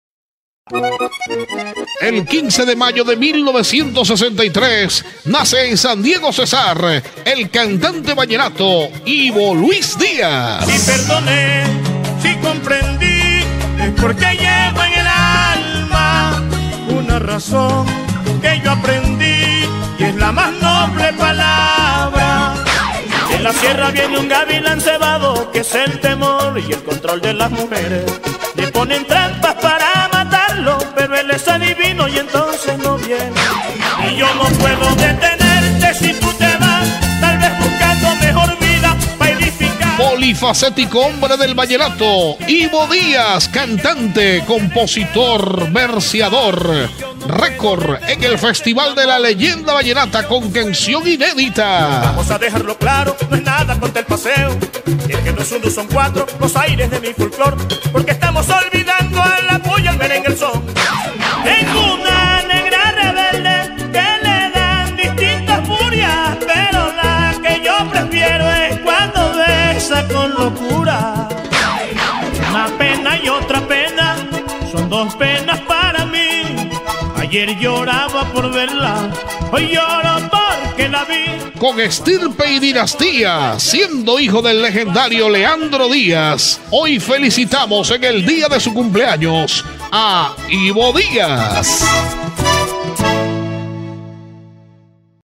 Ivo Diaz. Cumpleaños. Mayo 15.
El 15 de mayo de 1963 nació en sandiegocesar el cantante, verseador y compositor del vallenato @ivodiazlavoz
Hijo de la leyenda @leandrodiazoficial leandrodiaz record con 6 canciones inéditas ganadoras en el festivaldelaleyendavallenata @fesvallenato y un buen número de producciones discográficas al lado de grandes acordeoneros.
Feliz Cumpleaños